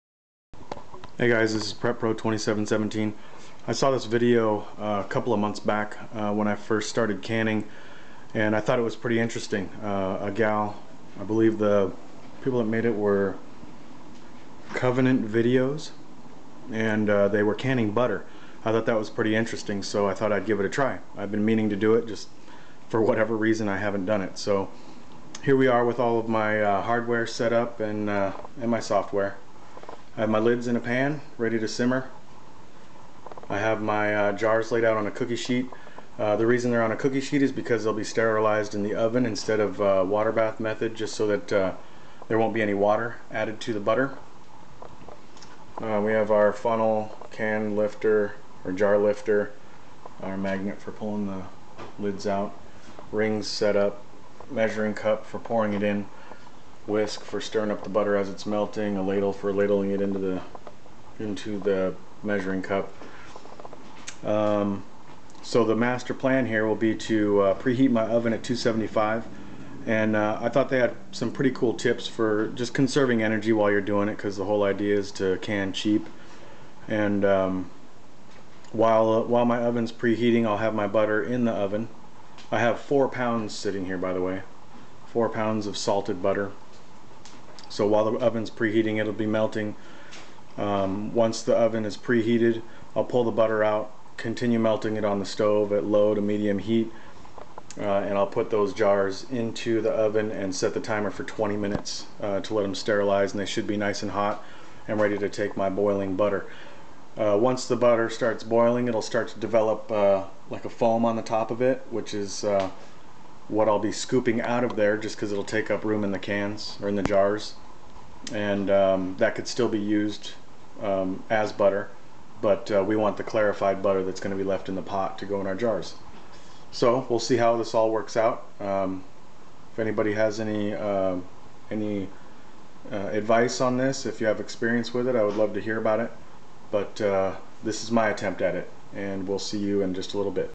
Canning butter pt.1
I saw a video a few months ago that walks you through canning clarified butter. I've been meaning to try it, but haven't taken the time. Until today.